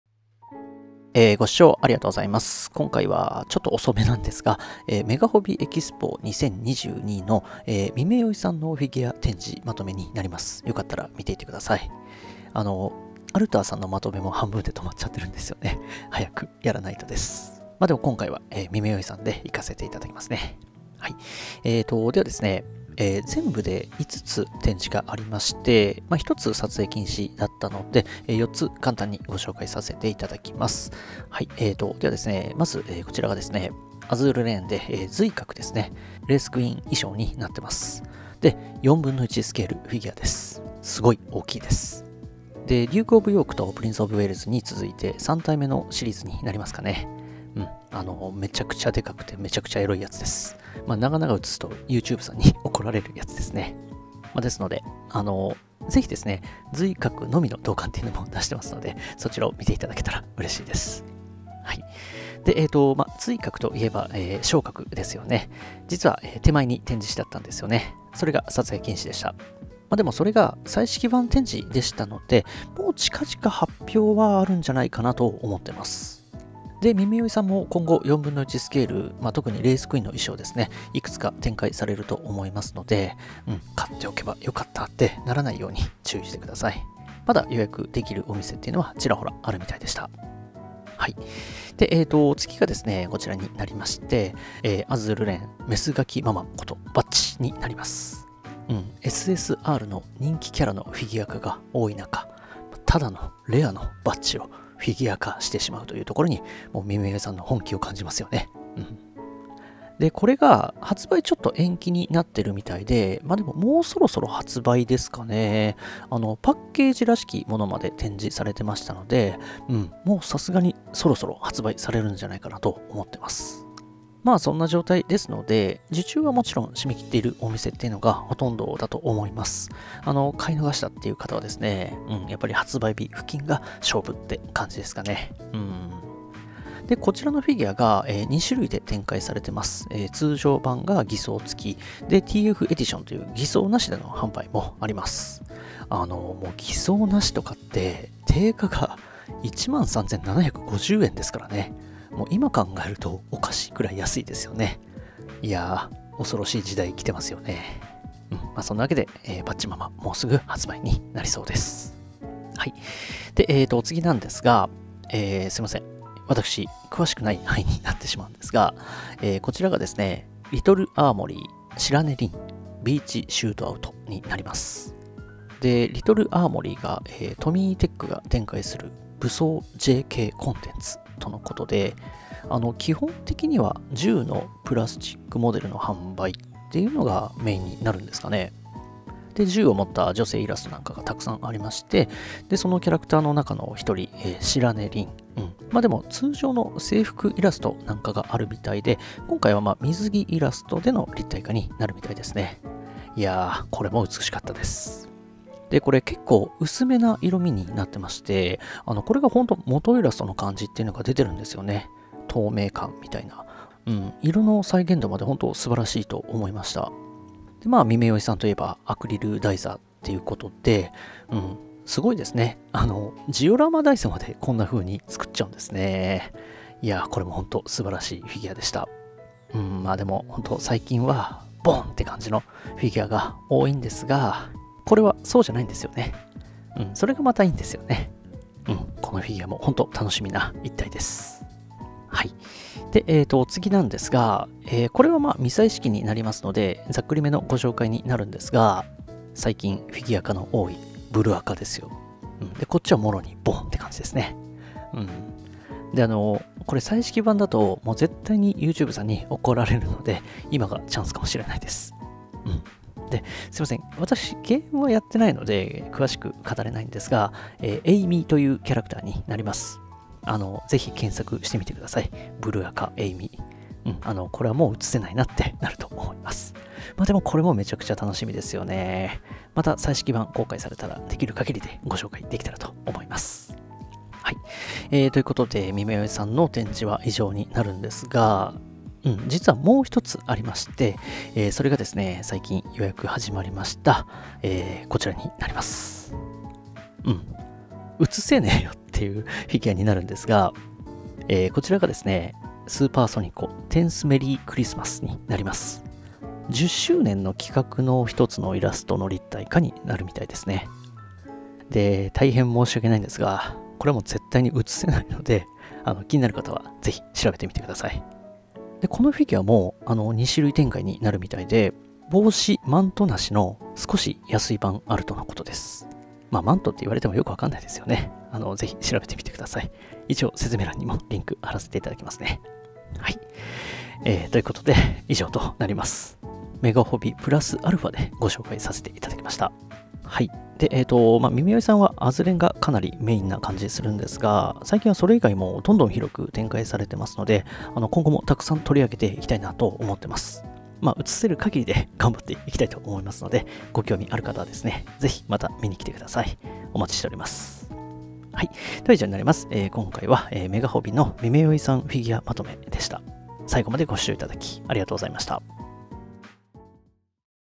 メガホビ EXPO2022 NEW DOT ミメヨイ フィギュア展示まとめ
amazonページ

アズールレーン バッチ 1/7スケール PVC&ABS製塗装済み完成品フィギュア

アズールレーン 瑞鶴 瞬速の真名 1/4スケール PVC&ABS製塗装済み完成品フィギュア

【公式】すーぱーそに子　 1♡th Merry Christmas !